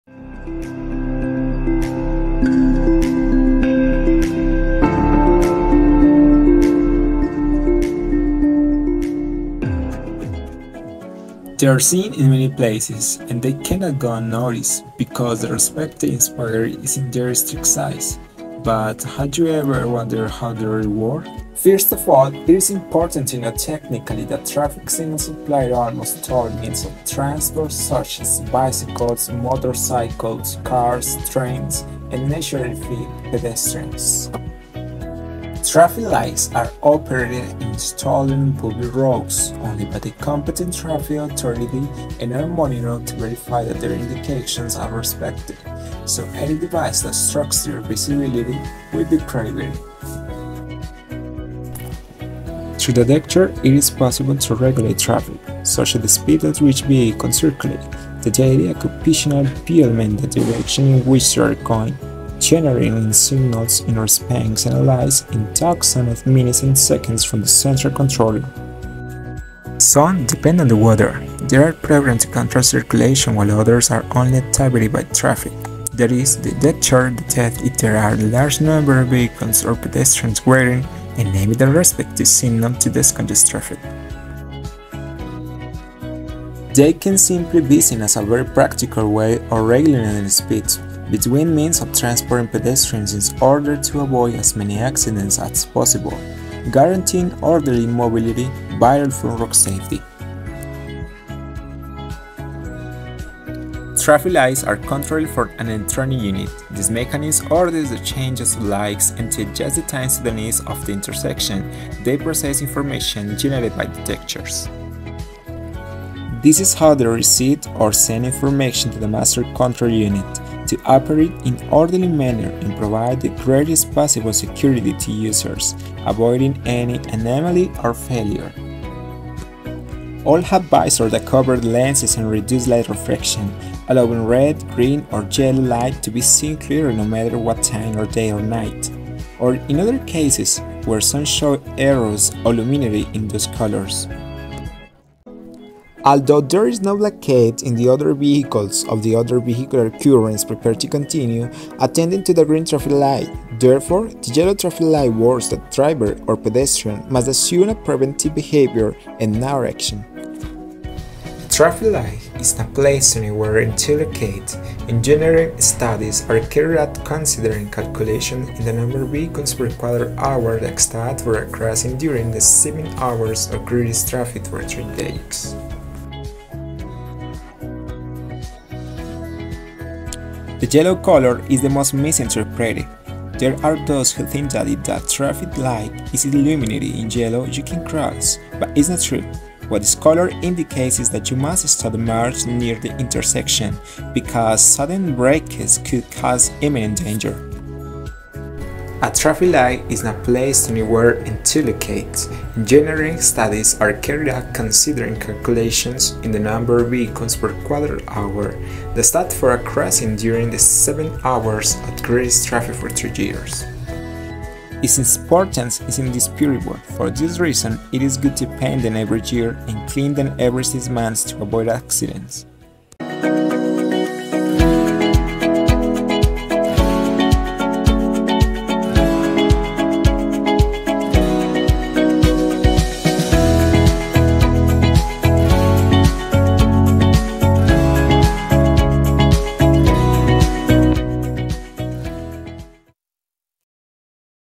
TRAFFIC LIGHT: FUNCTIONALITY
Join us and discover these signaling mechanisms and what is beyond emitting simple colors!